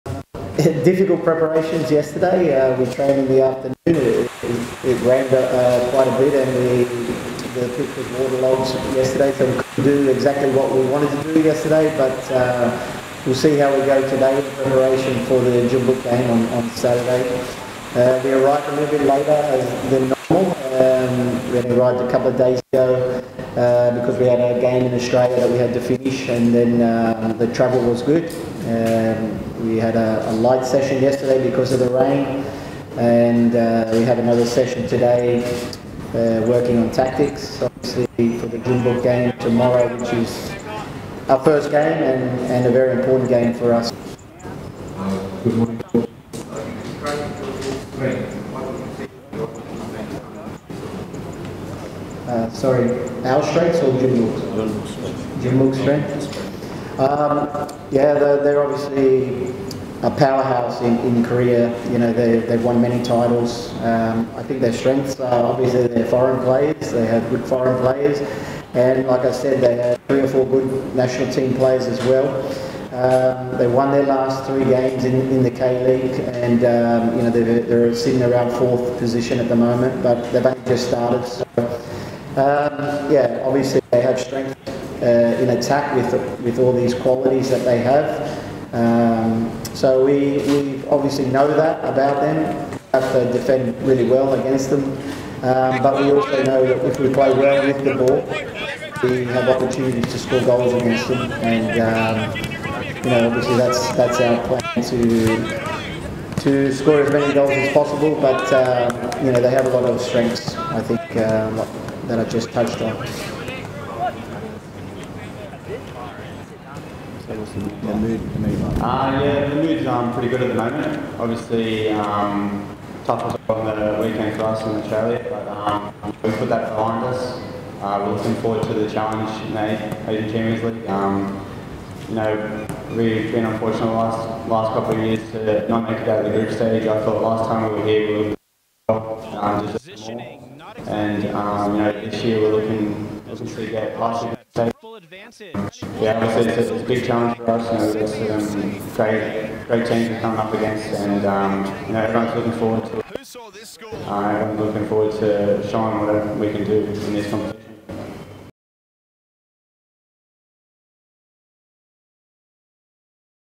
We've Deserved More From Champions League - Buhagiar
🎥 | “We’ve deserved more in previous Champions League campaigns” - Buhagiar.

@Sydney FC's leading scorer in @AFC Champions League history Trent Buhagiar believes the Sky Blues are determined to put it right this time, ahead of tonight's clash.